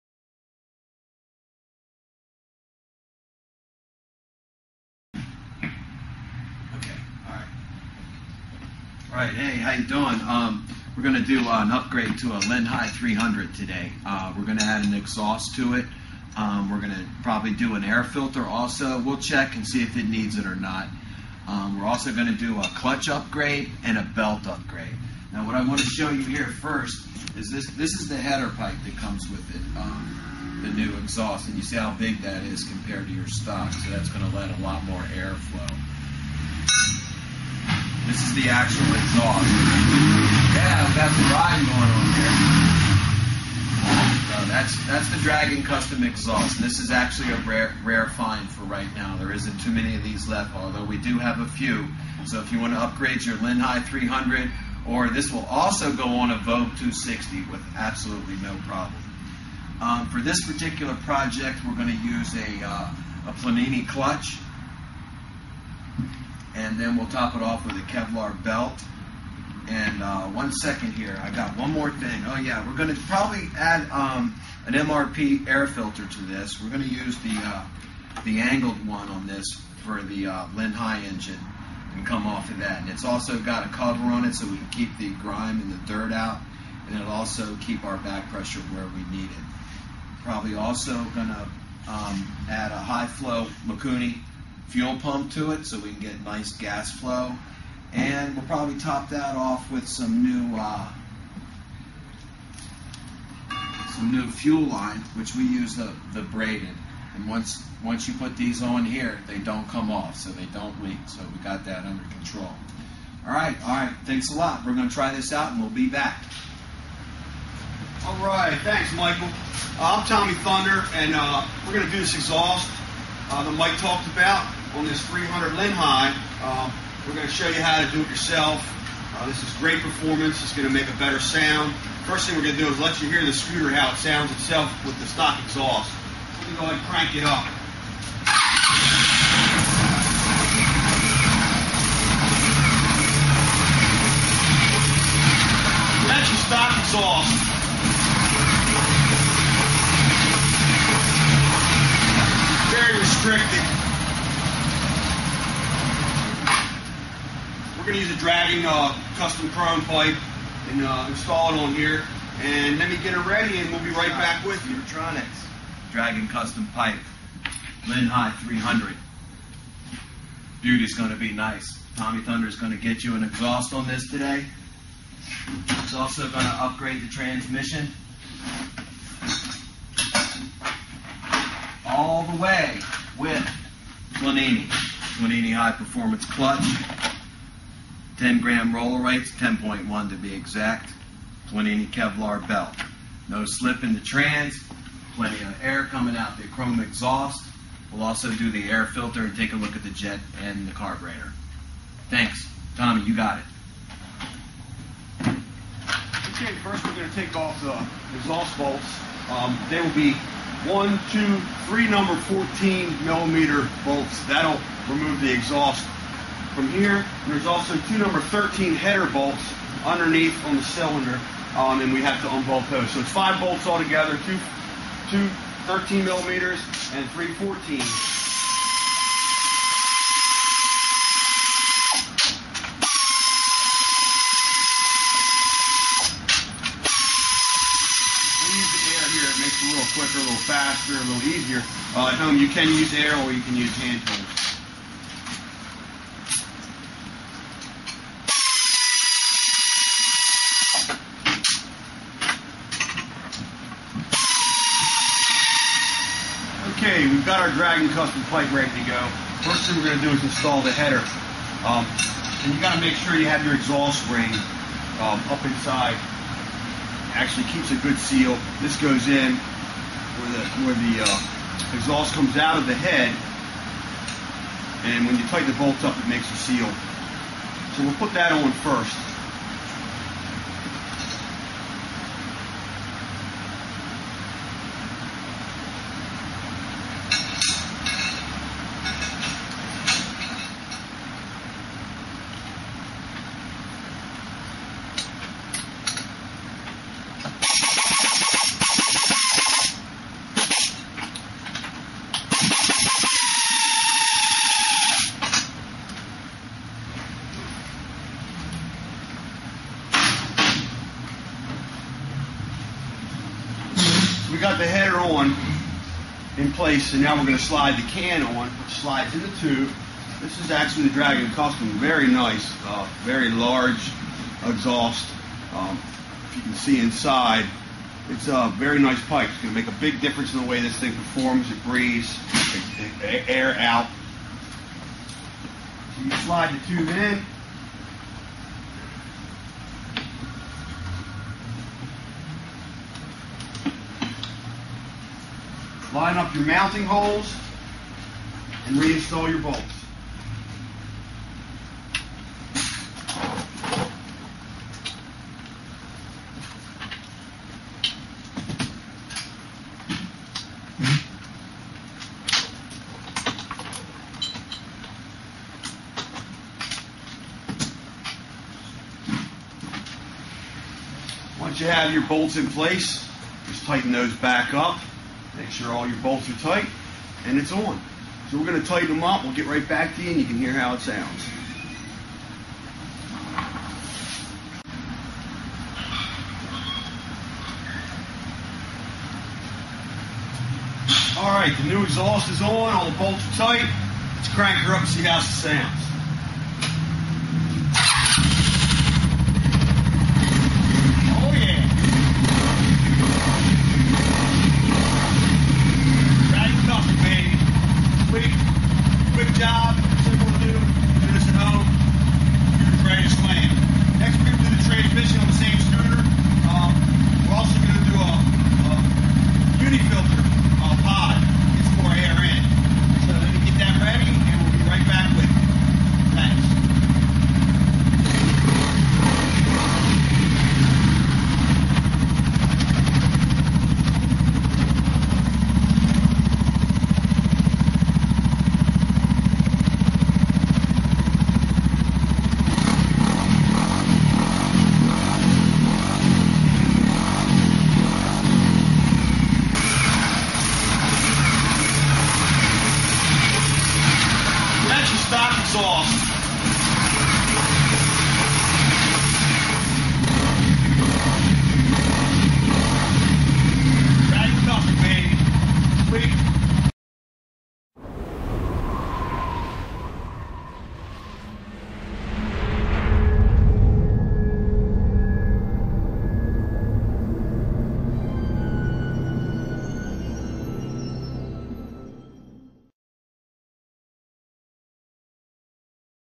Linhai 300 High Performance Upgrade on Exhaust
This video will discuss Part one of our high performance Linhai Aeolus 300 upgrades. Scootertronics has developed our own way of getting more performance form the Linhai 300 with exhaust along with gas flow upgrades with Mikini fuel pumps, high grade fuel line plus racing air filters plus jets and carbs, we put an OKO 28MM CARB ON ONE OF THESE WITH GREAT RESULTS, but no auto choke so its hard for customers, we did it to see if we could drag race one of these puppies.